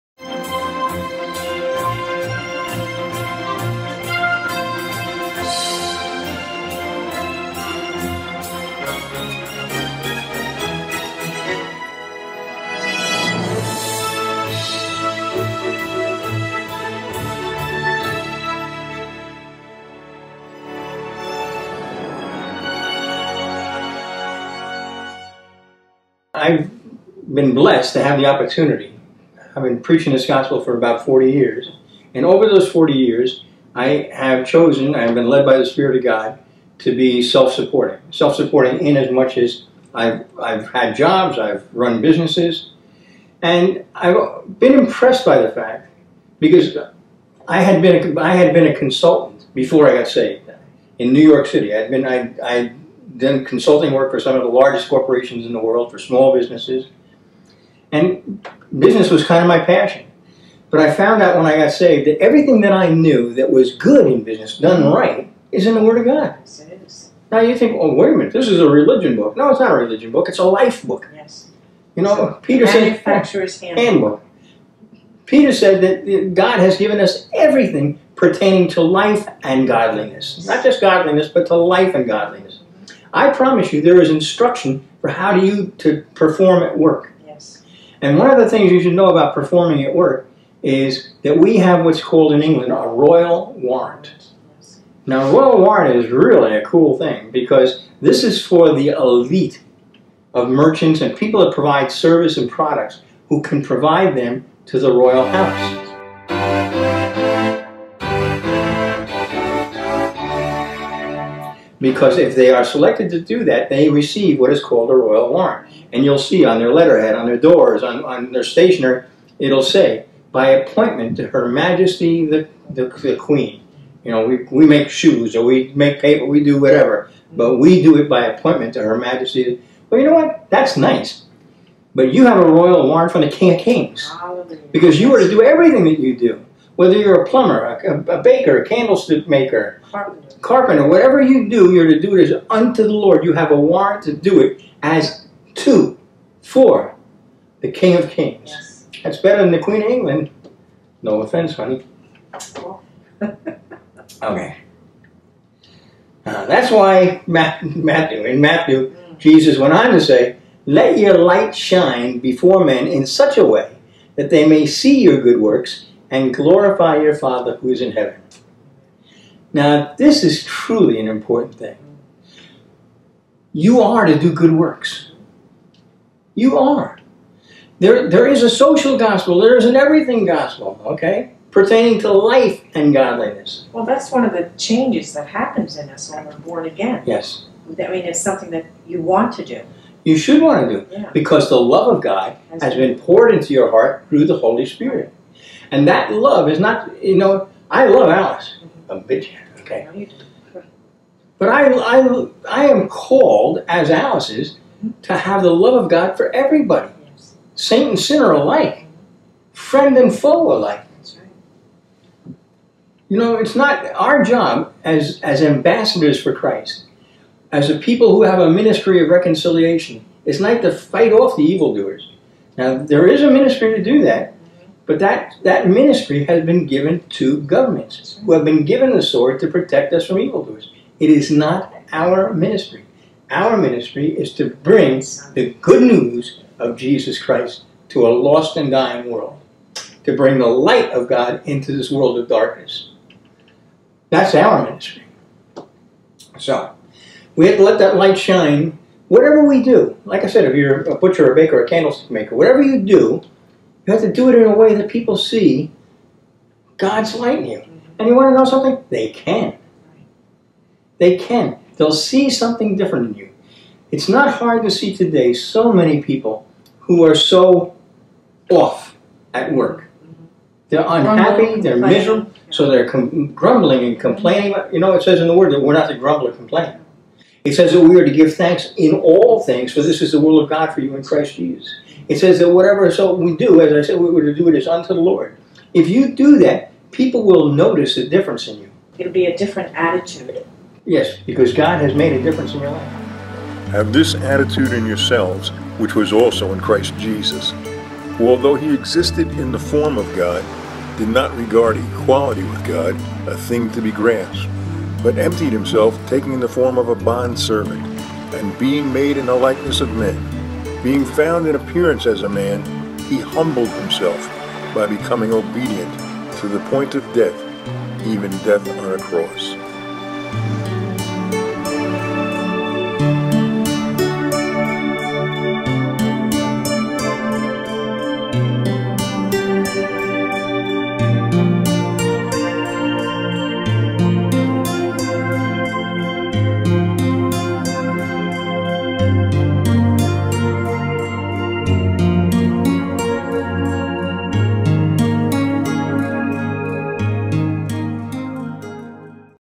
A Royal Warrant
Even more important than what you do, is who you do it for. In the United Kingdom there is no greater honor than to serve the Queen and receive a Royal Warrant. In the Kingdom of God we get to serve the King of Kings—we have a royal warrant and oh what a glorious blessing and privilege it is!